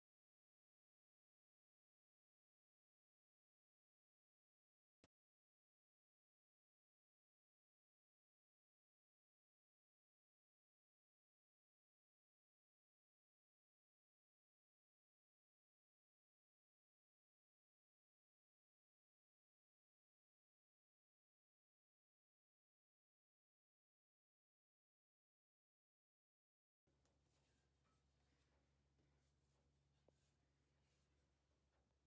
The Woolly Thistle's first yarn, Rambler is back is stock!
Made in NY state with American wool from the Southern Adirondack Fiber Producers Cooperative, Rambler is a woolly 3-ply fingering weight wool. 100% wool and suitable socks and so much more!